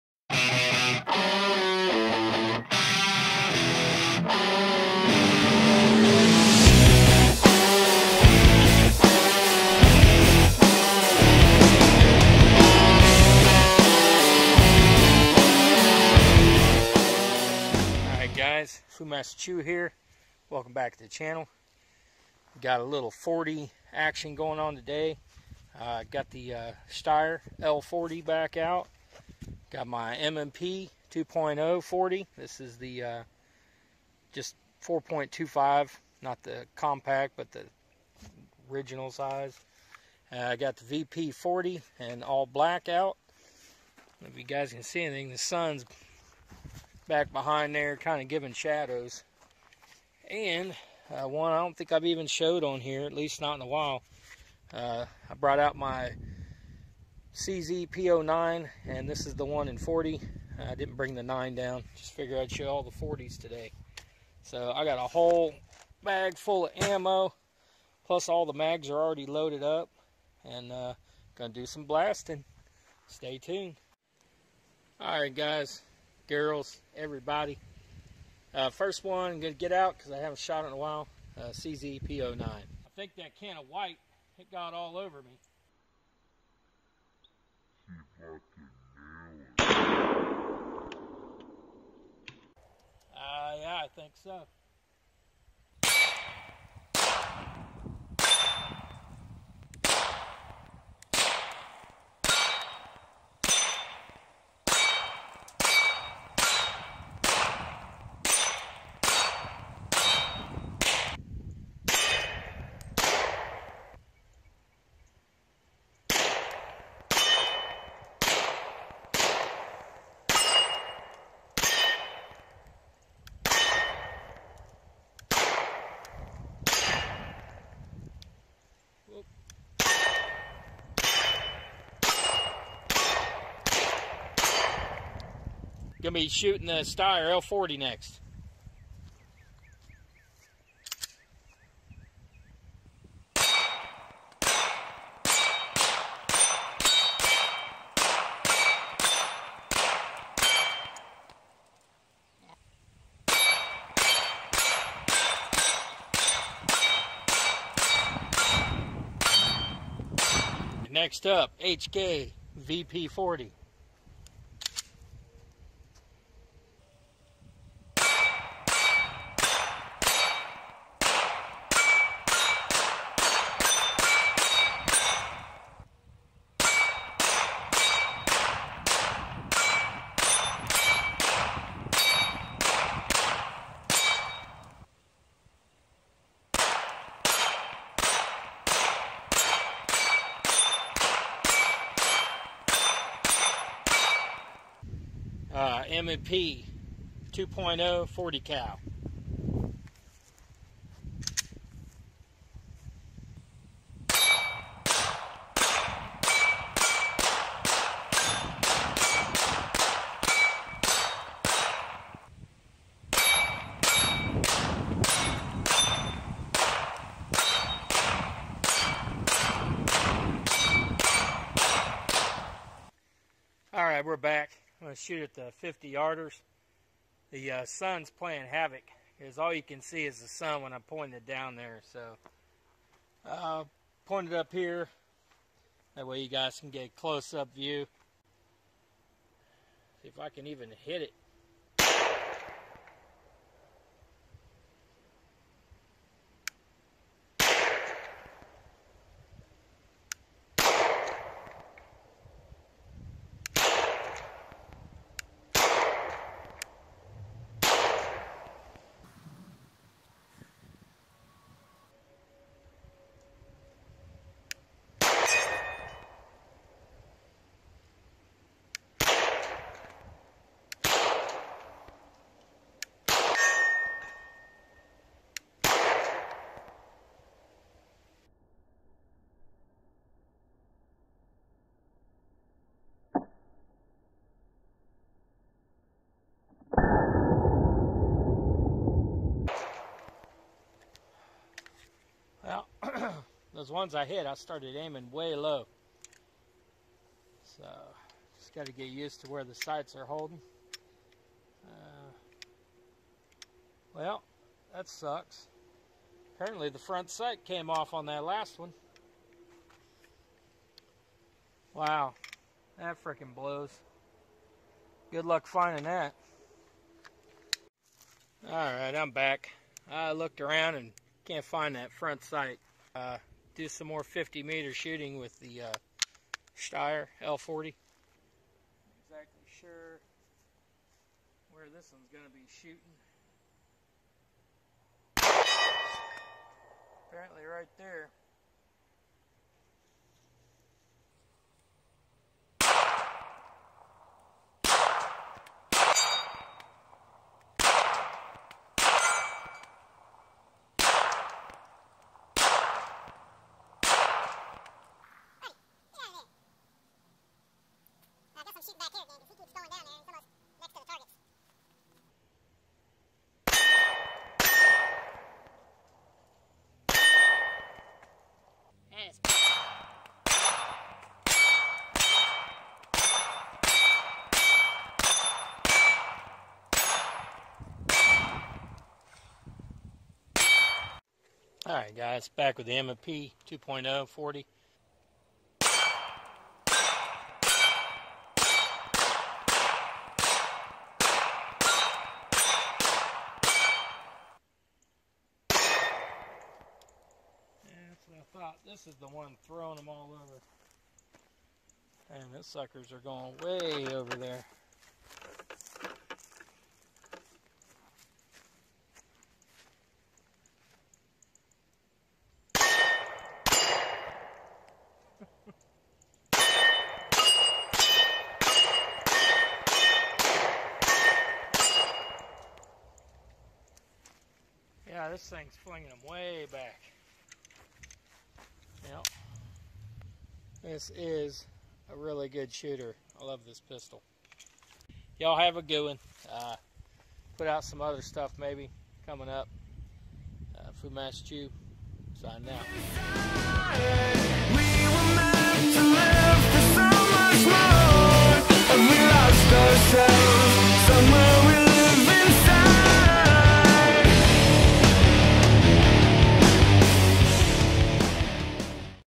HKVP40, Steyr L40, CZ P09 40, S&W M&P40 2.0 Forty caliber shoot-a-rama!
Had an ALL 40 caliber shootarama a few weeks ago and thought I'd upload the footage. Unfortunately I had a casualty during the shooting, it was the first time this sort of thing ever happened to me. Hope y'all enjoy, have a great weekend.